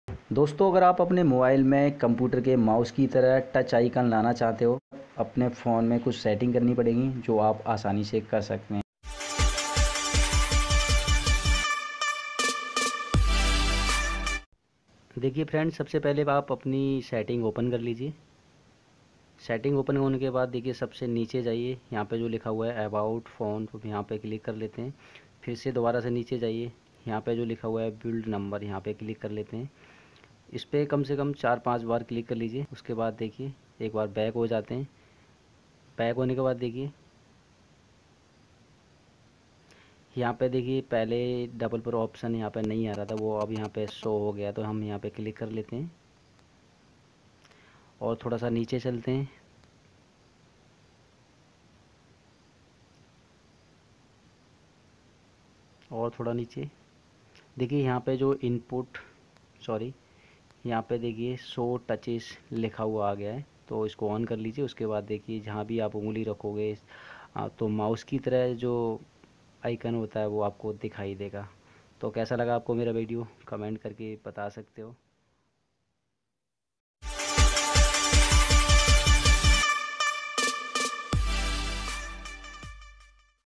How to use touch icon hindi.. Mobile me mouse ki tarah touch icon kaise ata hai
dosto  es vedeo me maine mobil  touch icon ki seting karana sikaya hai . kaise ham bina otg  kebal or bina mouse  ke bhi mobil me mouse jaisa touch icon la sakate hai asa kara ta hoo ki me aop ki hindi me help kar sakoo      daya technical hindi me jankari